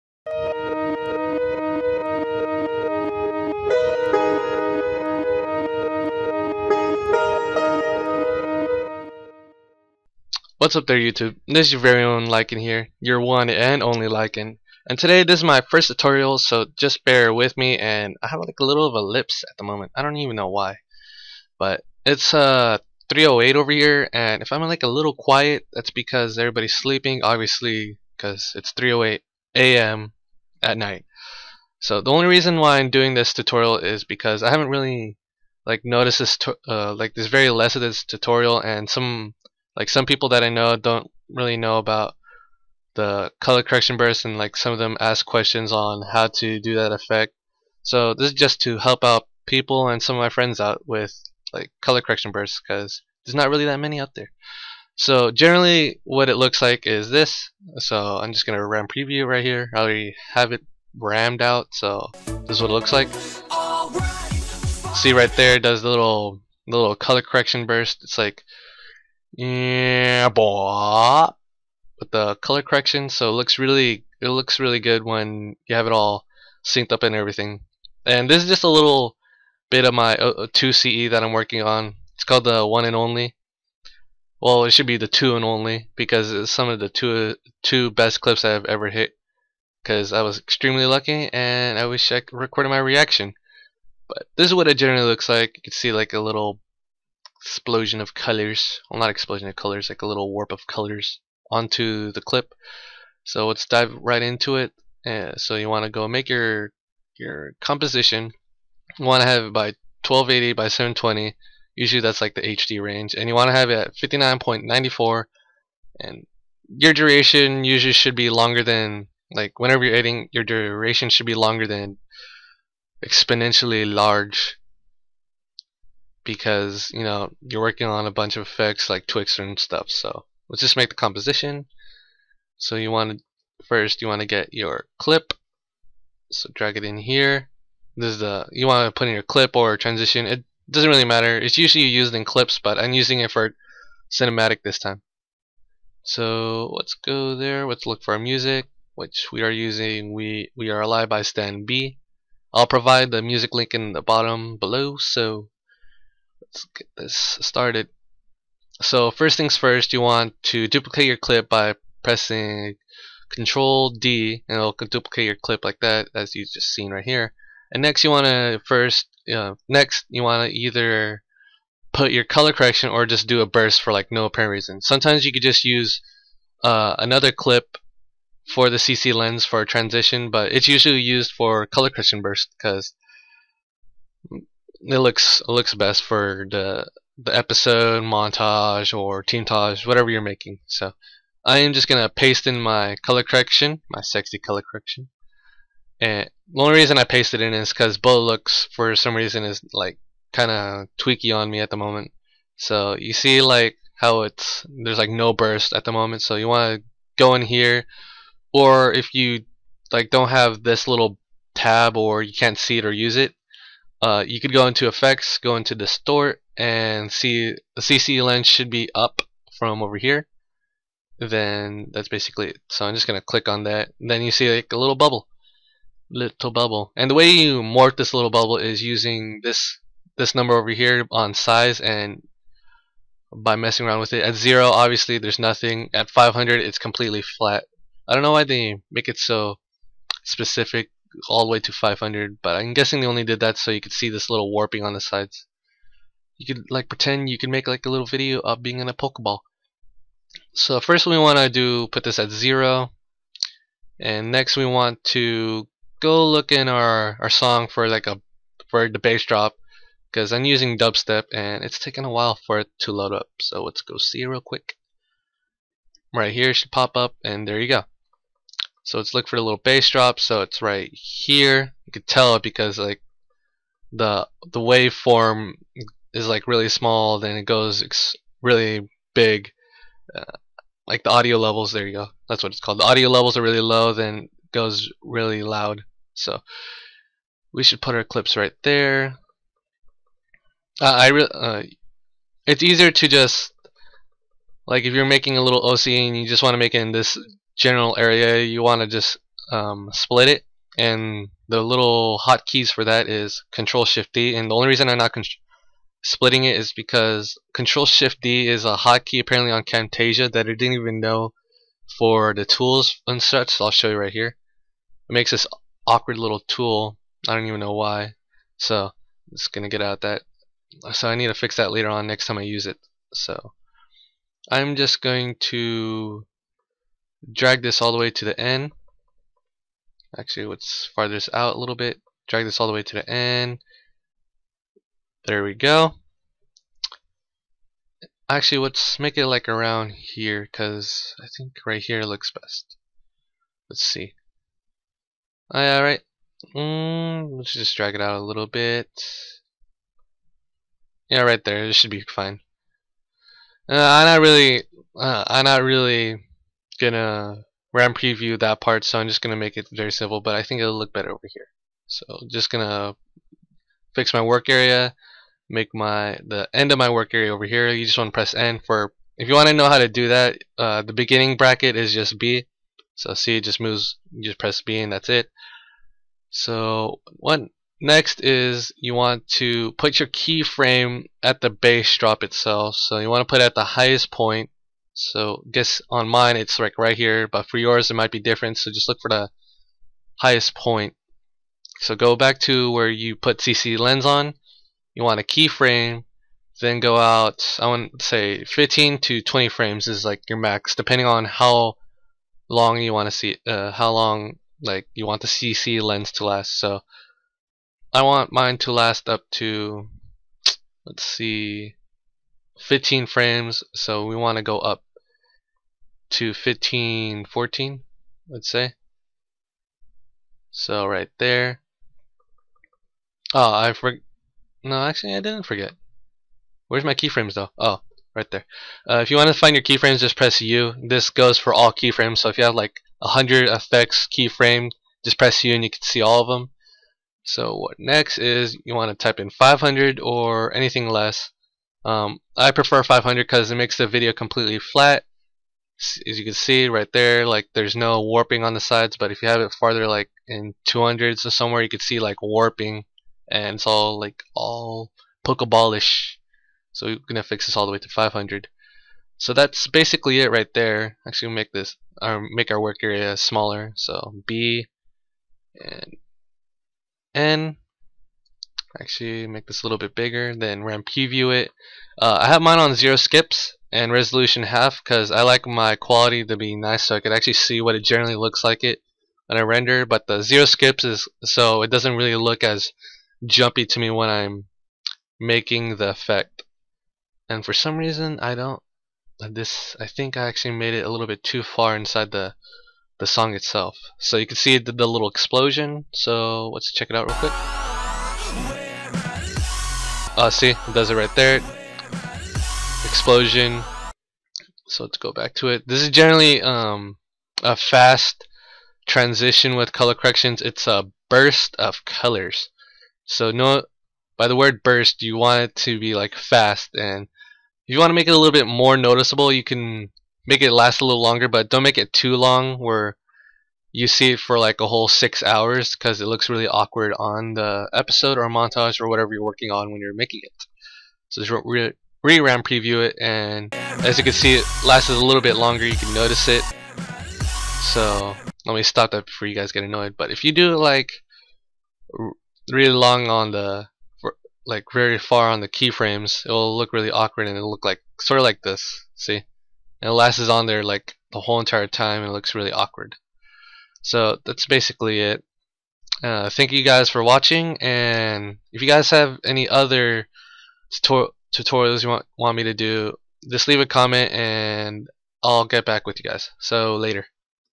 After Effects CS6 Tutorial: (color correction burst) First tut
Hey guys this is lyn and this is my first tutorial I basically did it on a simple effect but in the end other people still dont know about this effect so I generally made it for the people who need a little help with some effect so leave a like a sub and dont forget to comment for another tut
For the people disliking my video's IDGAF still gunna make vids because its alot of fun so there is no stopping a beast